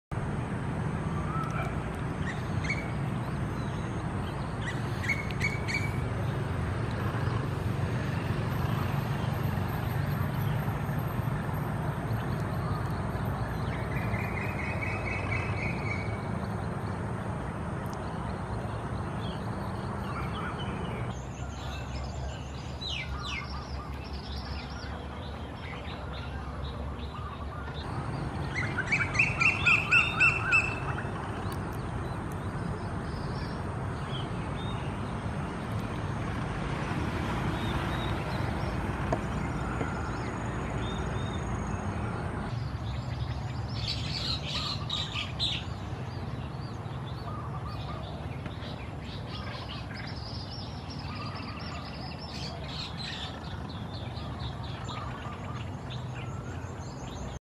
แมงมุมแมวป่ากินแมลงปอเข็ม Lynx spider eating a damselfly
แมงมุมแมวป่าชวา(Java Lynx spider) Oxyopes javanus Thorell, 1887กินแมลงปอเข็มเล็กคาดเขียว Agriocnemis minima (Selys, 1877) ที่บ้านเขาย้อย ต.เขาย้อย อ.เขาย้อย จ.เพชรบุรี วันที่ 14 ธันวาคม 2559